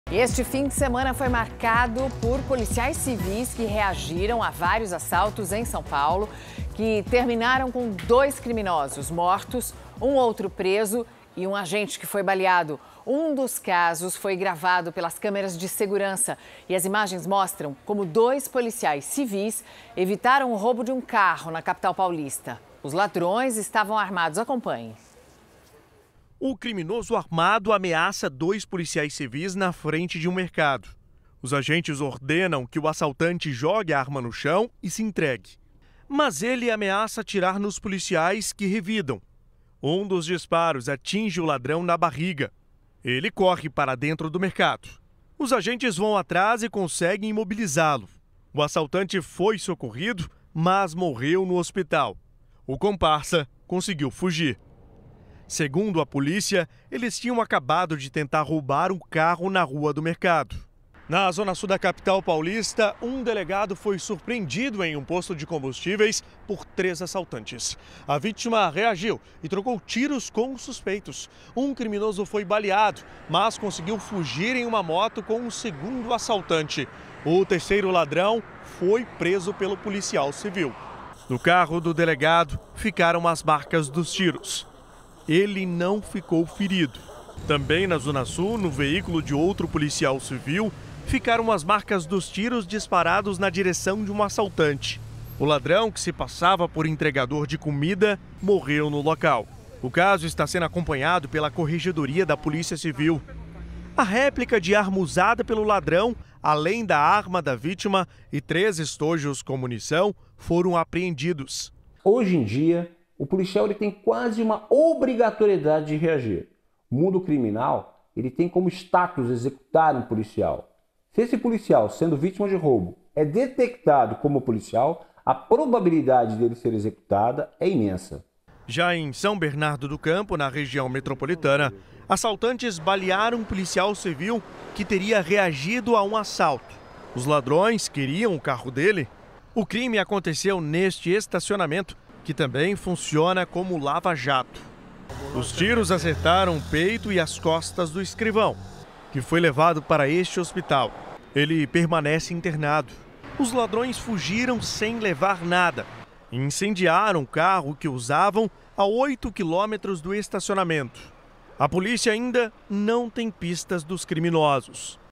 Policiais reagem a assaltos e matam dois bandidos em SP
O fim de semana em são Paulo foi marcado pelas reações de policiais civis a assaltos que terminaram com dois criminosos mortos, outro preso e um agente baleado. Um dos casos foi gravado por câmeras de segurança. As imagens mostram como dois policiais civis evitaram o roubo de um carro na capital paulista. Os ladrões estavam armados.